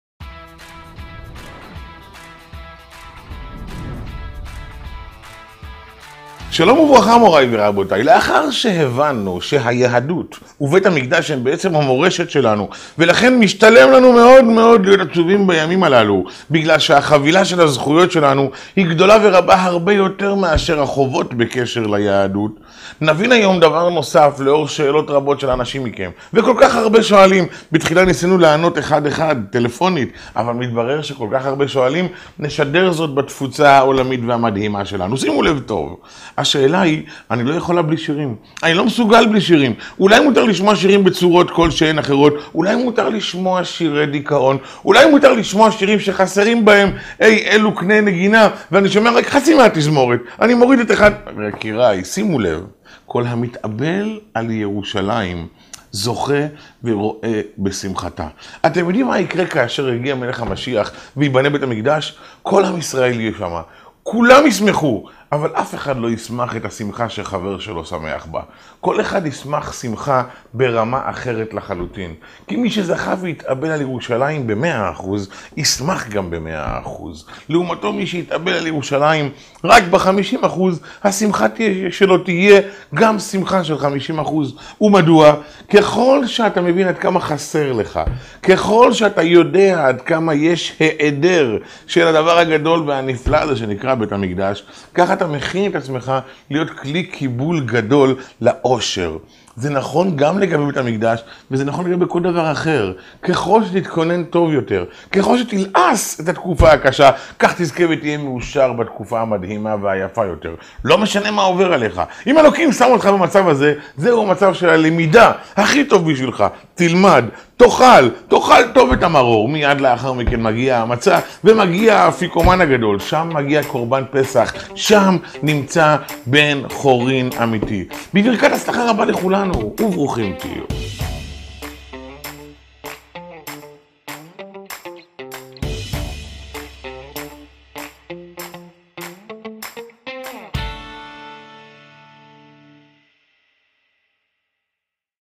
🎧האם שיר טוב הוא חלק מחוויית השמחה? האם ניתן להתלהב דוקא מיום ללא שיר? 🎧Is a good song part of the
🎧האם שיר טוב הוא חלק מחוויית השמחה? האם ניתן להתלהב דוקא מיום ללא שיר?
🎧Is a good song part of the joy? It is possible to get excited about a day without song?

👇🏻 _*לחצו על הקישור והצטרפו:*_
*Join us, just click:*

🖇  *_צרפו לקבוצות:_*
📡שתפו - Share📡